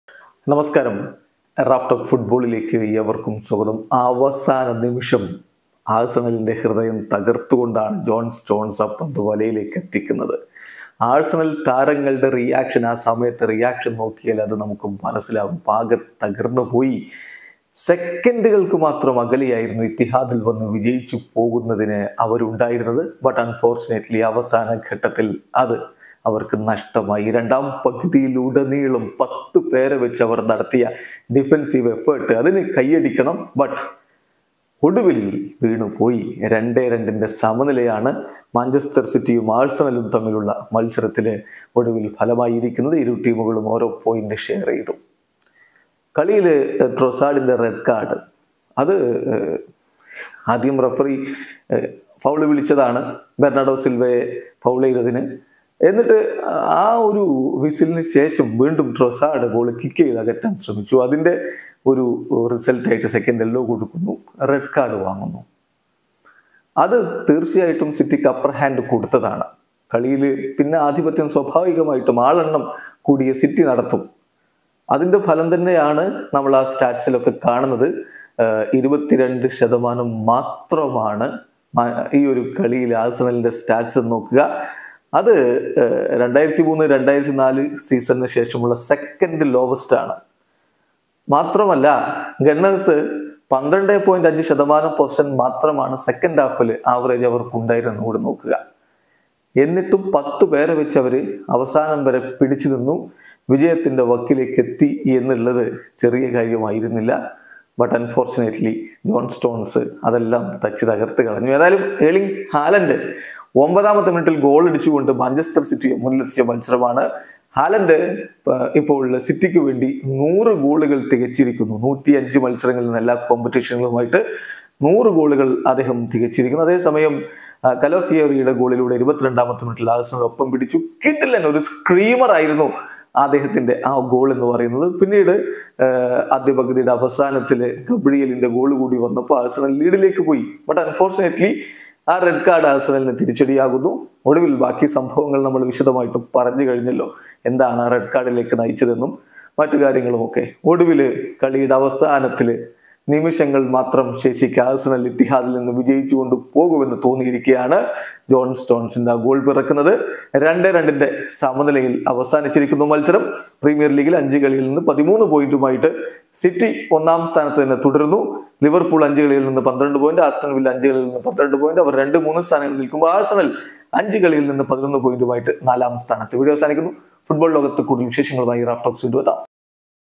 അവസാന നിമിഷം ആഴ്സണലിൻ്റെ നെഞ്ച് തകർത്ത് സ്റ്റോൺസ്, ഇത്തിഹാദിൽ സമനില | Manchester City vs Arsenal
Manchester City vs Arsenal
Premier League